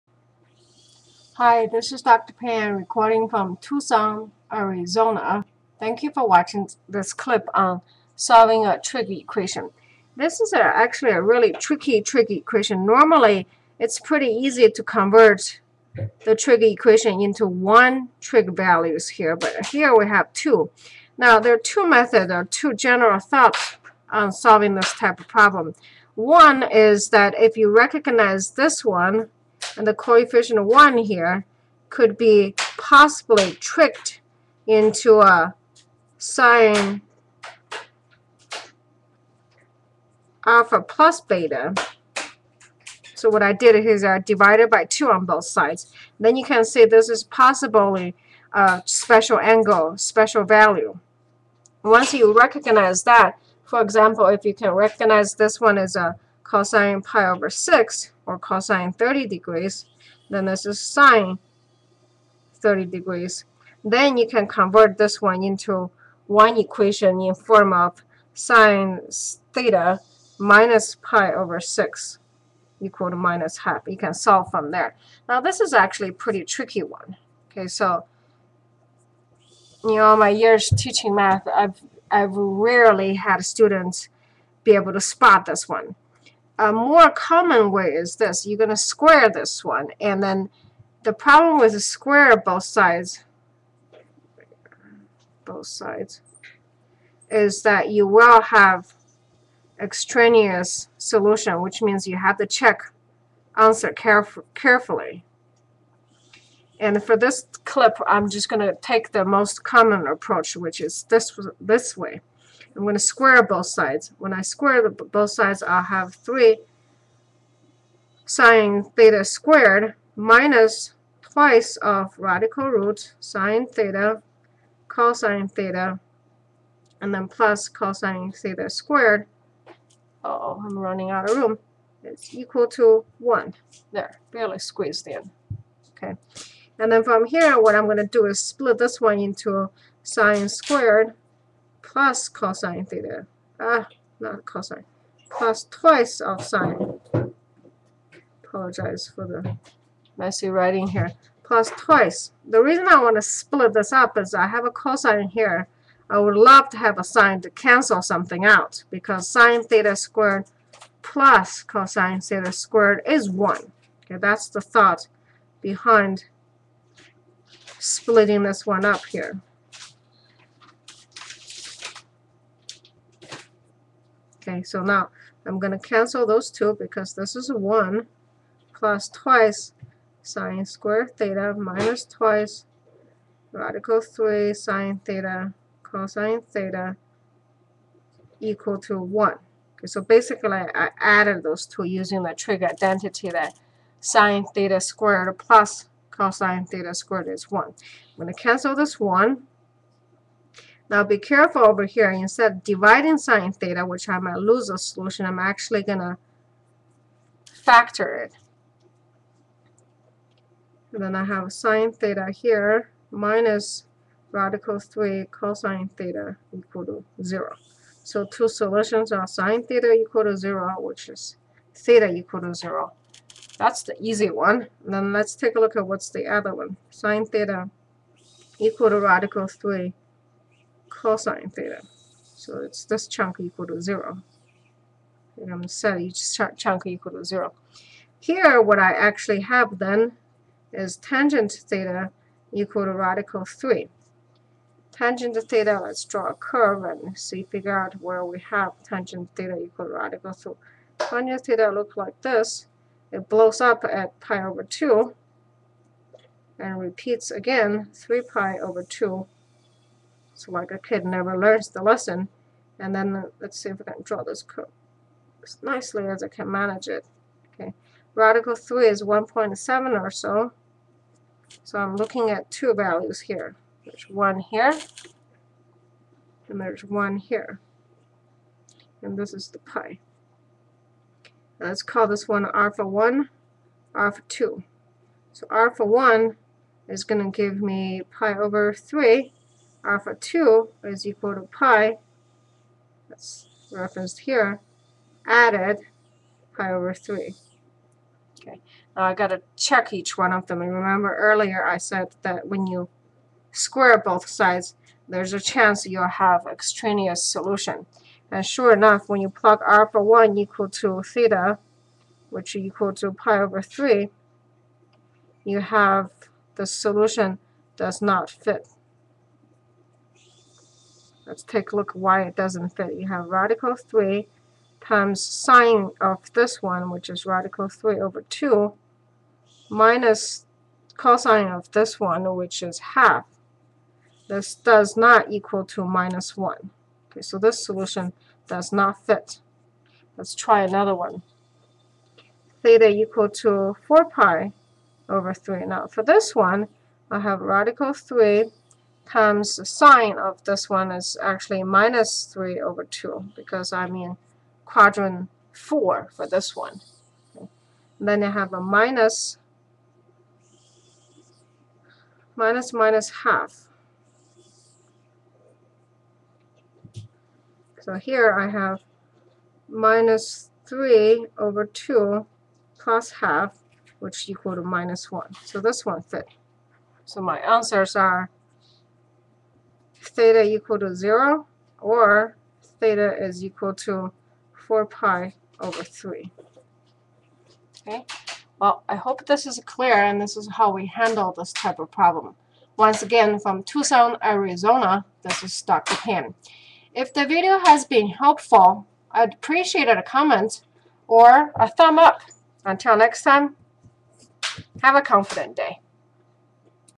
*Trig Equation: √3 sinθ-cosθ = -1, θ on the interval between 0 and 2pi
A production.Solve √3 sinθ-cosθ = -1, θ on the interval between 0 and 2pi